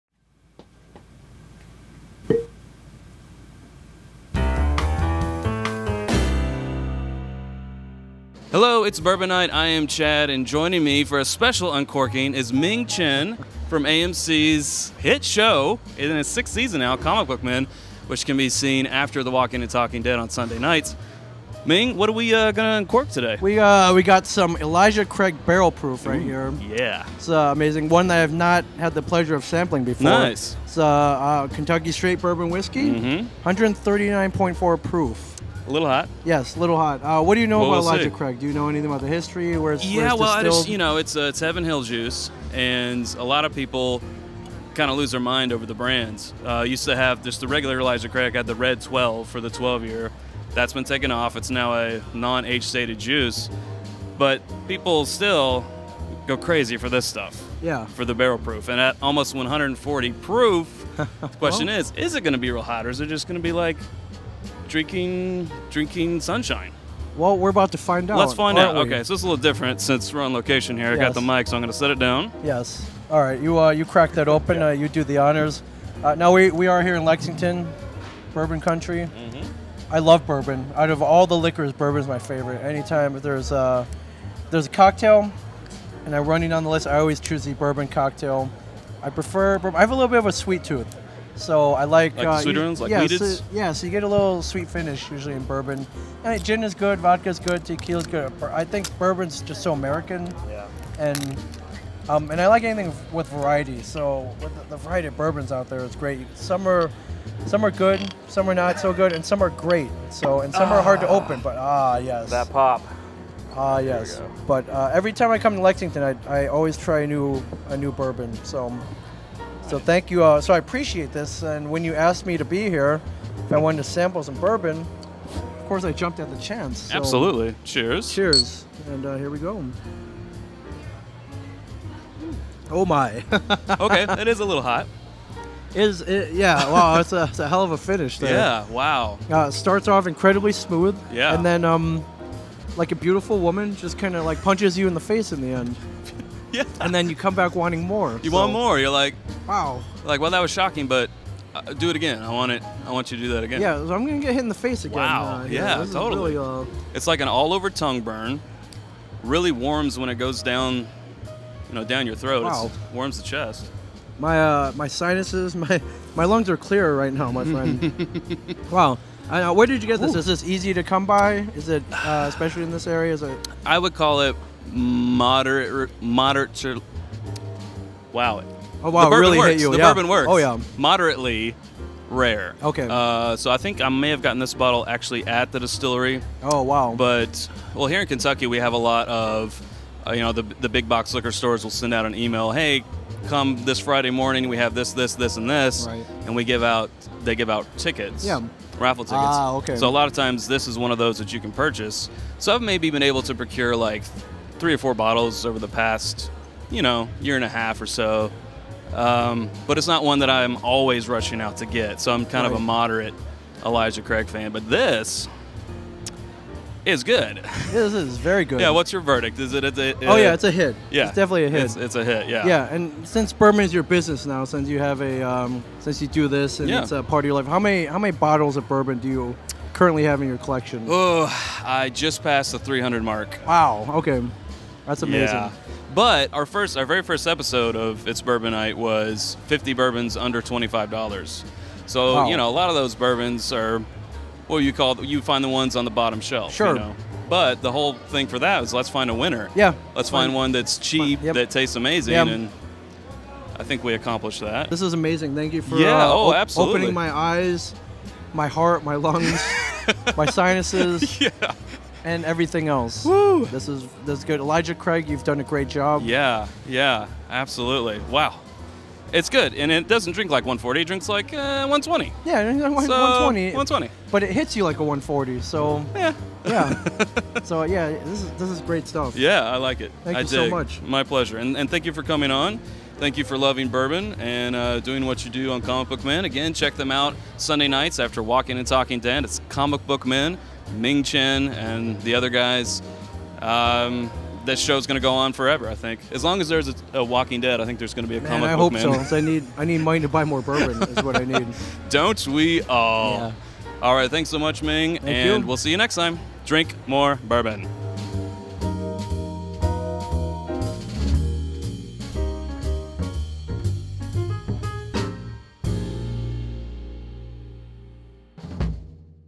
Uncorking Elijah Craig Barrel Proof Bourbon - 139.4 proof!
Chad cracks open a bottle of Elijah Craig Barrel Proof at 139.4 proof with Ming Chen from AMC's hit show "Comic Book Men".

Follow Ming on Twitter and Instagram @MingChen37

Like the glasses and other items we use for Flight Fights,
Uncorkings and more?